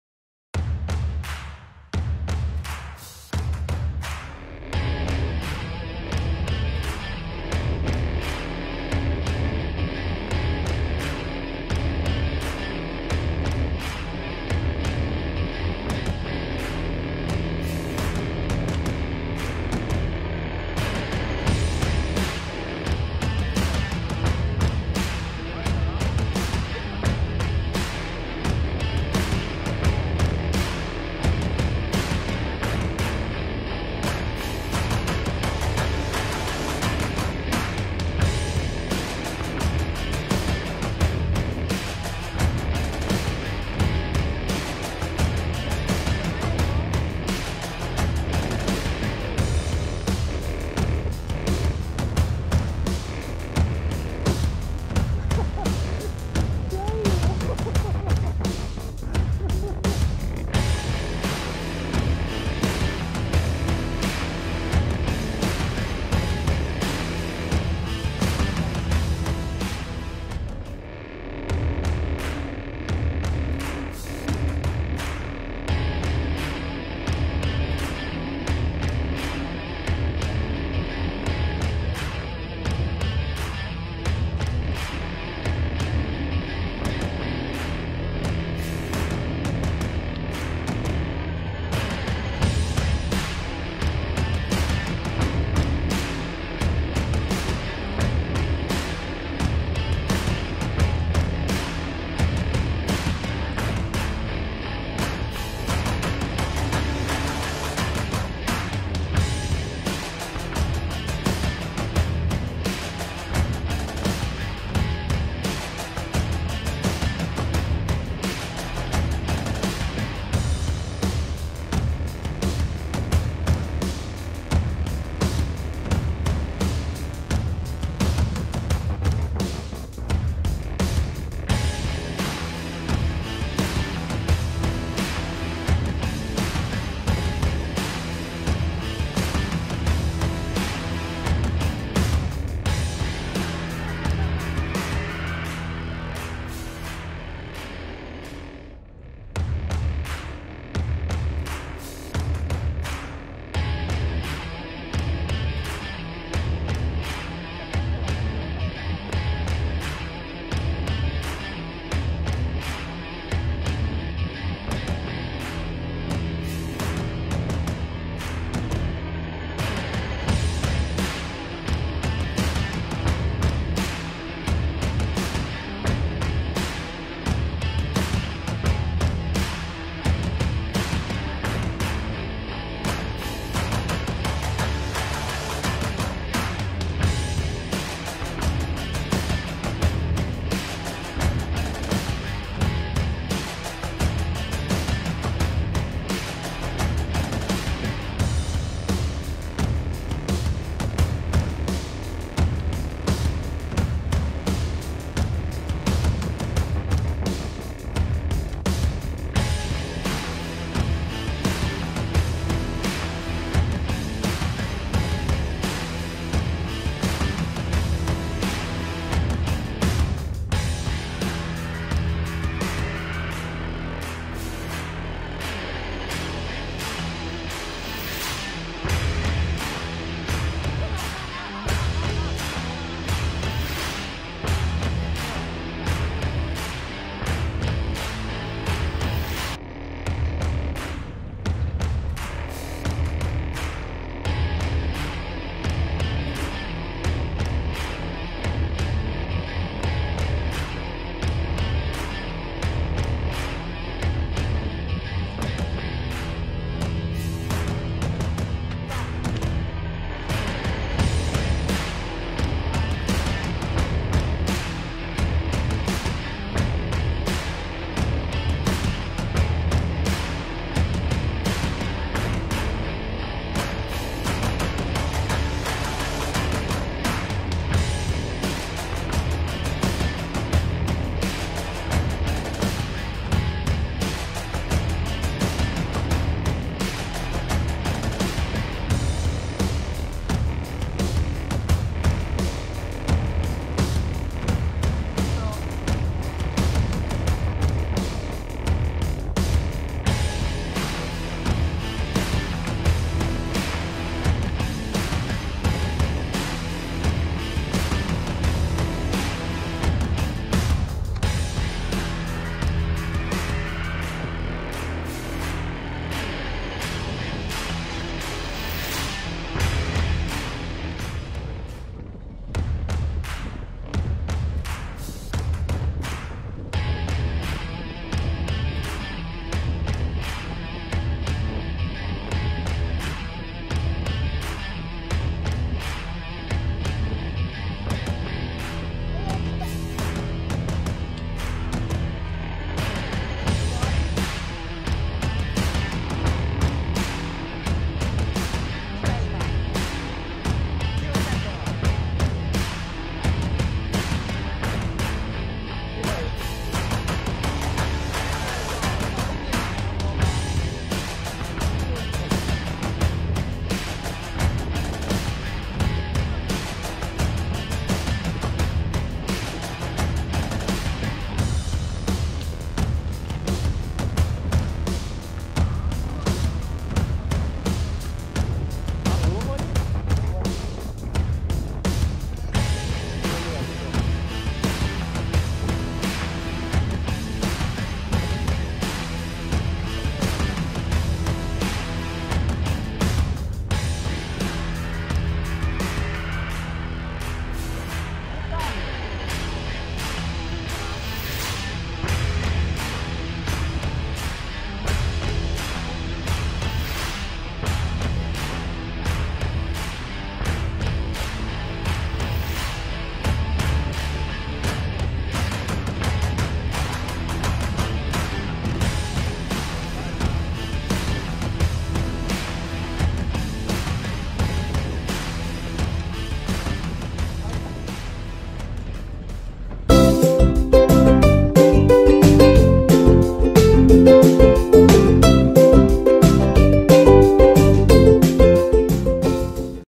【日記用】2022.5.3 久里浜〜剱崎冲マダイ 間口港 日の出丸
ブログ用挿絵動画です。
※How to系ではありません。ただ釣りをしているだけの動画です。